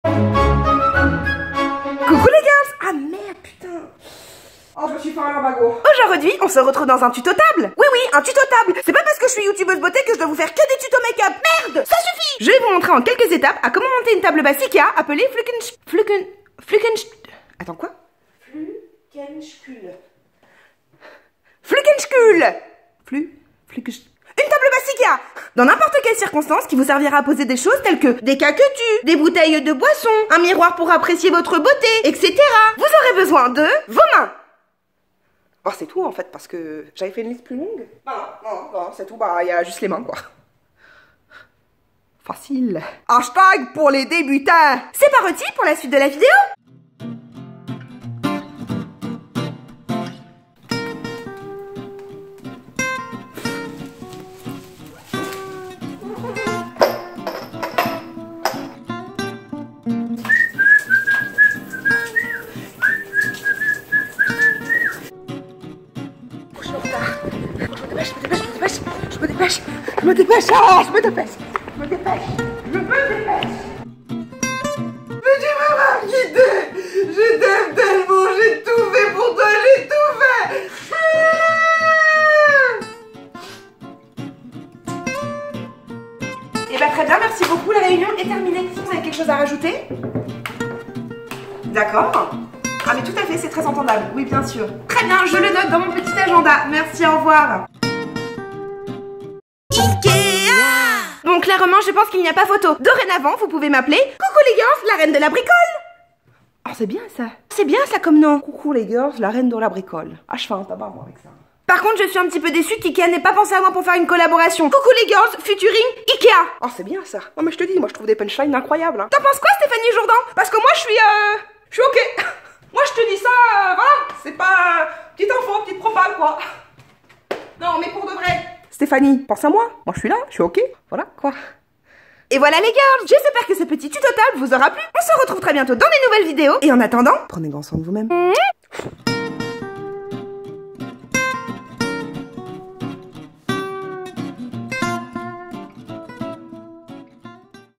♡ JE SUIS LA REINE DE LA BRICOLE ♡
Coucou les girls !
Aujourd’hui on se retrouve dans un tuto table !
J’avais envie de sortir de ma zone de confort et vous montrer que le bricolage et moi ça fait vraiment 1 !  ❤️👷‍♀️🔧
LOVE

MATERIEL /
Je filme avec un Nokia 3310